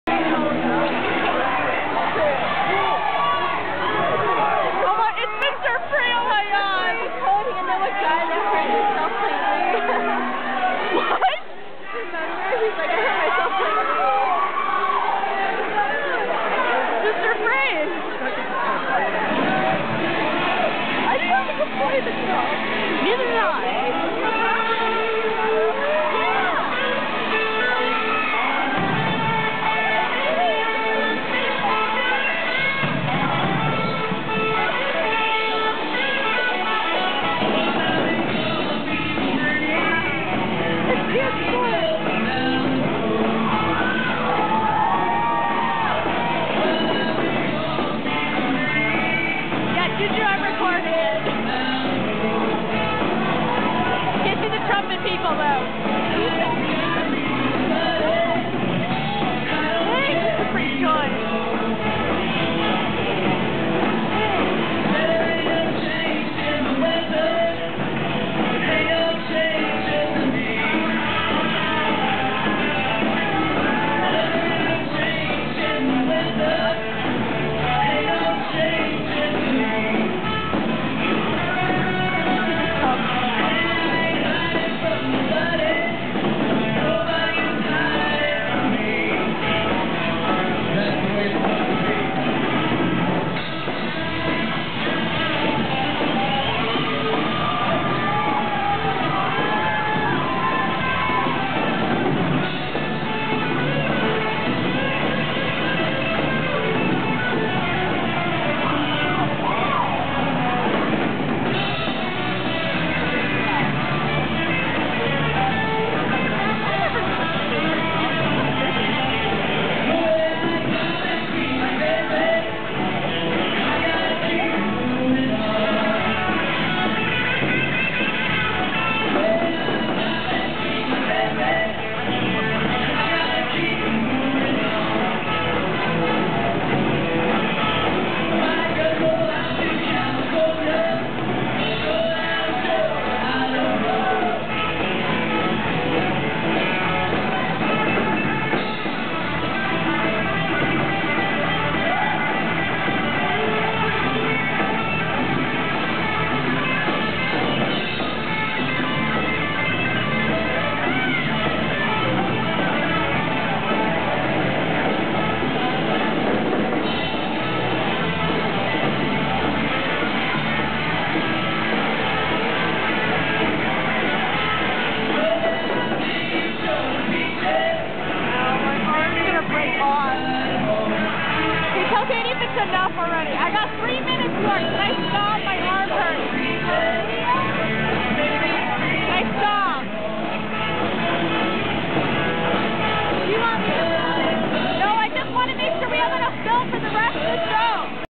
G-Street Band Grant High School V-Show 2008
This is one of the songs (that I do not know which one it was) that G-Street Band played